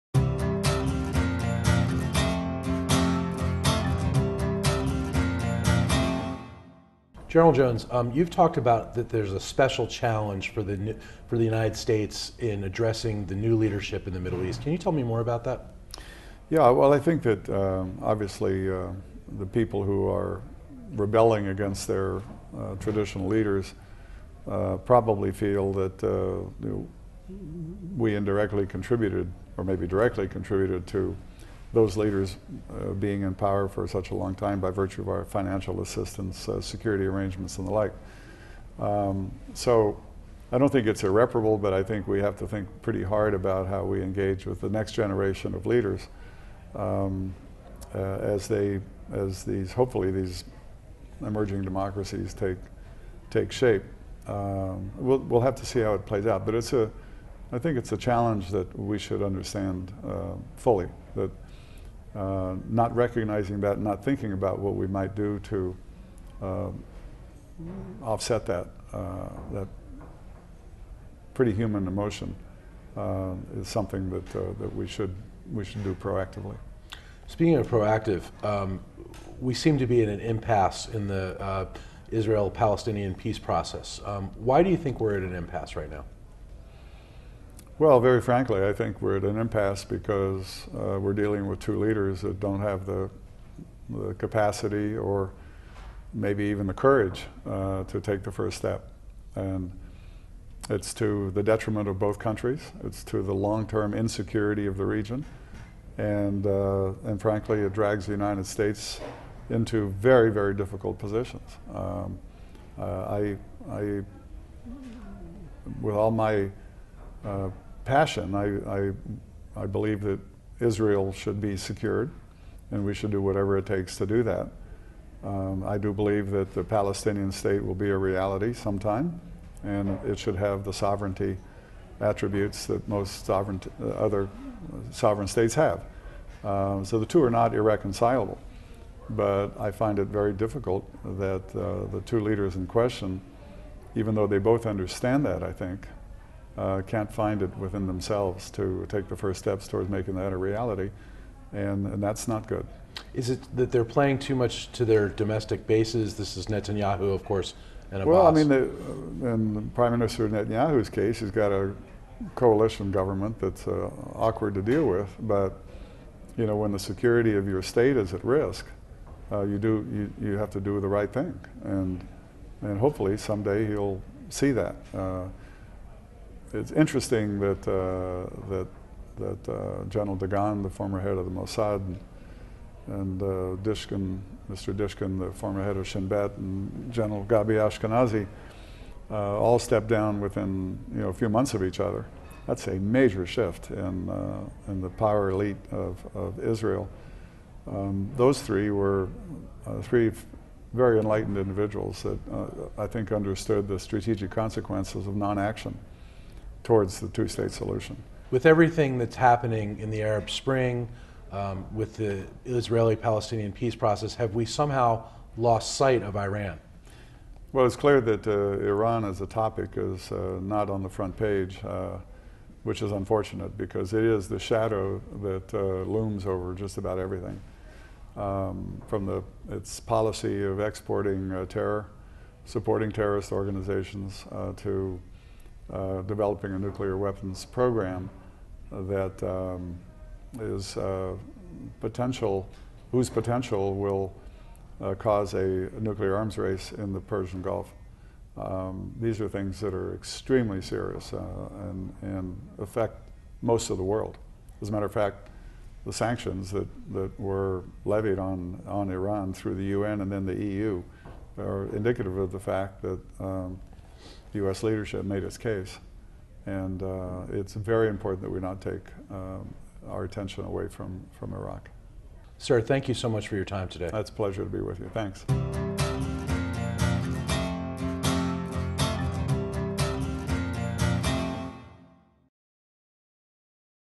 Middle East Leadership with General James L Jones
General James L. Jones, former National Security Adviser, and H. Andrew Schwartz, CSIS senior vice president for External Relations, spoke at the CSIS Global Security Forum 2011 about the lack of leadership in the Middle East.